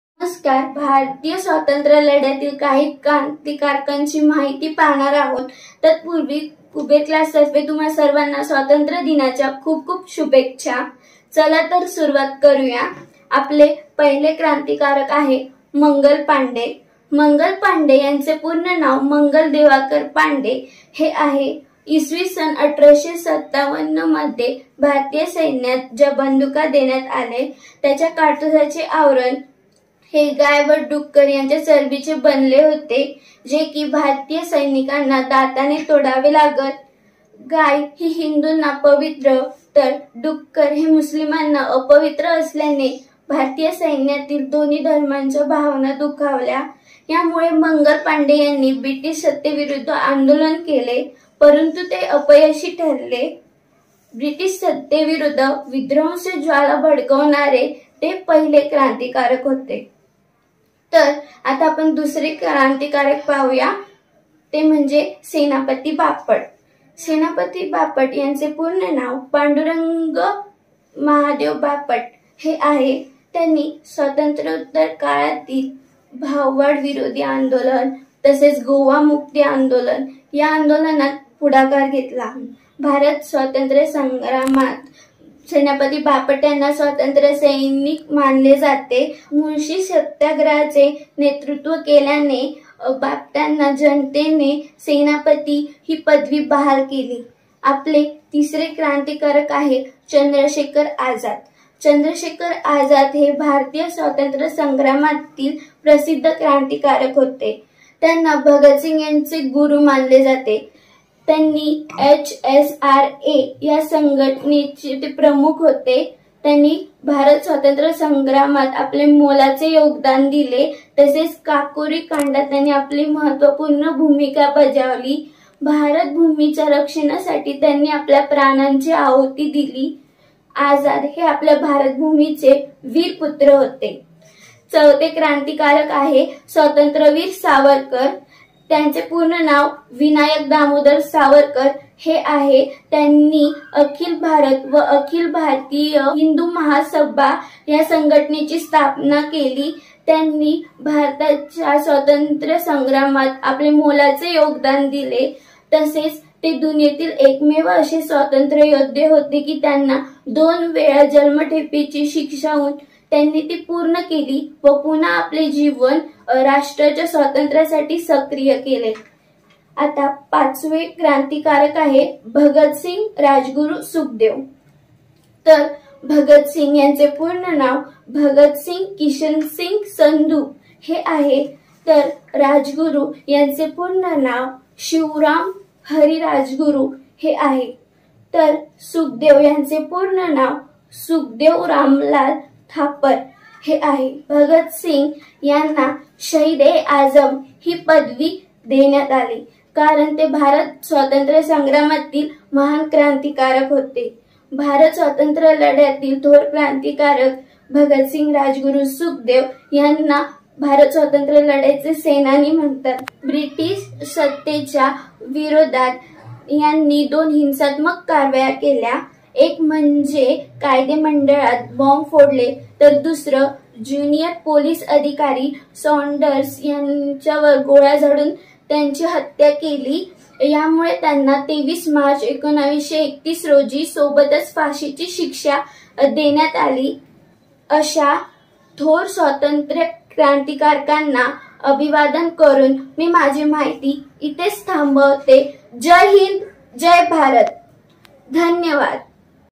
भारतीय क्रांतिकारकांची माहिती- Bhartiya Krantikarak Marathi Mahiti/Indian Revolutionaries In Marathi
🇮🇳भारतीय क्रांतिकारकांची ओळख🇮🇳
आजच्या व्हिडीओ मध्ये आपण "थोर भारतीय क्रांतिकारक"/- "Great Indian Revolutionaries"- यांची माहिती मराठी मध्ये पहाणार आहोत.

पहिले "क्रांतिकारक मंगल पांडे"
( Krantikari Mangal Pande )👇
मंगल पांडे यांचे पूर्ण नाव मंगल दिवाकर पांडे हे आहे. इ.स. १८५७ मध्ये भारतीय सैन्यास ज्या बंदुका देण्यात आल्या त्याच्या काडतुसाचे आवरण हे गाय व डुकराच्या चरबीचे बनलेले होते. जे की भारतीय सैन्याला दाताने तोडावे लागत. गाय ही हिंदूंना पवित्र व डुक्कर हे मुस्लिमांना अपवित्र असल्याने भारतीय सैन्यातील दोन्ही धर्माच्या भावना दुखावल्या गेल्या. त्यामुळे Mangal Pande यांनी पुढाकार घेऊन ब्रिटिश सत्तेविरुद्ध आंदोलन केले परंतु ते अपयशी ठरले. इंग्रजां विरुद्ध विद्रोहाची ज्वाला भडकवणारे मंगलपांडे हे पहिले क्रांतिकारक होते.

दुसरे "क्रांतिकारक सेनापती बापट"
( Krantikari Senapati Bapat )👇
सेनापती बापट यांचे नाव पांडुरंग महादेव बापट हे आहे. स्वातंत्र्योत्तर काळातील भाववाढ विरोधी आंदोलन, गोवामुक्ती आंदोलन यात सेनापतीबापट यांनी पुढाकार घेतला. सेनापतीबापट हे भारतीय स्वातंत्र्य संग्रामातील स्वातंत्र्य सैनिक होते. मुळशी सत्याग्रह याचे नेतृत्व केल्यामुळे त्यांना जनतेने 'सेनापती' ही पदवी बहाल केली.

तिसरे "क्रांतिकारक चंद्रशेखर आझाद"
( Krantikari Chandrashekhar Aazad ) 👇
चंद्रशेखर आझाद हे भारतीय स्वातंत्र्य संग्रामातील प्रसिद्ध "krantikarak" होते. त्यांना भगतसिंग यांचे गुरू मानले जाते. एच. एस. आर. आय. या संघटनेचे ते प्रमुख होते. त्यांनी भारत स्वातंत्र्य समरात आपले महत्वपूर्ण योगदान दिले व 'काकोरी कांडात' आपली महत्वपूर्ण भूमिका बजावली. भारताच्या रक्षणासाठी चंद्रशेखर आझाद यांनी  आपल्या प्राणाची आहुती दिली. आझाद हे भारत देशाचे वीर पुत्र होते.

चौथे "क्रांतिकारक विनायक दामोदर सावरकर"
( Krantikari Swatantryavir Sawarkar )👇
त्यांना स्वातंत्र्यवीर असे म्हटले जाते. त्यांनी अखिल भारत व अखिल भारतीय हिंदू महासभा या संघटना स्थापन केल्या. स्वातंत्र्यवीर सावरकर यांनी भारत स्वातंत्र्य लढ्यात मोलाची भूमिका पार पाडली. ते दुनियेतील एकमेव असे स्वातंत्र्य योद्धा होते की ज्यांना दोन वेळा जन्मठेपेची शिक्षा झाली असून ती पूर्ण केली व पुन्हा आपले जीवन राष्ट्र सेवेत सक्रिय केले.

पाचवे "क्रांतिकारक भगतसिंग-राजगुरू-सुखदेव" ( Krantikari-Bhagatsingh-Rajguru-Sukhdev )👇
भगतसिंग यांचे पूर्ण नाव भगतसिंग किशनसिंग संधू हे आहे.
तर राजगुरू यांचे पूर्ण नाव शिवराम हरी राजगुरू हे आहे.
व सुखदेव यांचे पूर्ण नाव सुखदेव रामलाल थापर हे आहे.
Bhagat Singh यांना "शाहिद ए आजम" ही पदवी देण्यात आली. कारण ते भारत स्वातंत्र्य संग्रामातील महानक्रांतिकारक होते.
भारत स्वातंत्र्य संग्रामातील भगतसिंग राजगुरू सुखदेव हे तिन्ही विख्यात स्वातंत्र्य सेनानी होते.
हिंदुस्थानातील ब्रिटिश साम्राज्य वादाविरोधीच्या लढ्यात त्यांनी दोन हिंसात्मक कार्ये केली.
१) कायदेमंडळात बॉम्ब फोडले.
२) ज्युनिअर पोलीस अधिकारी सोंडर्स यांची गोळ्या झाडून हत्या केली.
यामुळे त्यांना २३ मार्च १९३१ रोजी फाशीची शिक्षा देण्यात आली. अशाप्रकारे "थोर भारतीय क्रांतीकारक" यांनी त्यावेळच्या स्वातंत्र्य लढ्याला सतत धगधगत ठेवले होते.
अशाप्रकारे "थोर भारतीय क्रांतिकारक" (Thor Bhartiya Krantikarak / Krantikari), यांनी भारत देशाला स्वातंत्र्य देण्यासाठी अनमोल प्रयत्न केले, अशा "Indian Great Revolutionaries" (थोर भारतीय क्रांतिकारक) यांच्या कार्याला सलाम!!! 🙏
(आपला आभारी आहे, धन्यवाद 🙏)
TIMESTAMPS 👇

0:00 भारतीय क्रांतिकारकांची माहिती
0:14 पहिले "क्रांतिकारक मंगल पांडे"
1:02 दुसरे "क्रांतिकारक सेनापती बापट"
1:38 तिसरे "क्रांतिकारक चंद्रशेखर आझाद"
2:14 चौथे "क्रांतिकारक विनायक दामोदर सावरकर"
2:50 पाचवे "क्रांतिकारक भगतसिंग-राजगुरू-सुखदेव"

इंग्रजीतील शब्दांच्या जाती हा विषय 5 मिनिटात सोप्या पद्धतीने समजून घ्यायचा असेल तर १००% खालील लिंकवर प्रेस करा 👇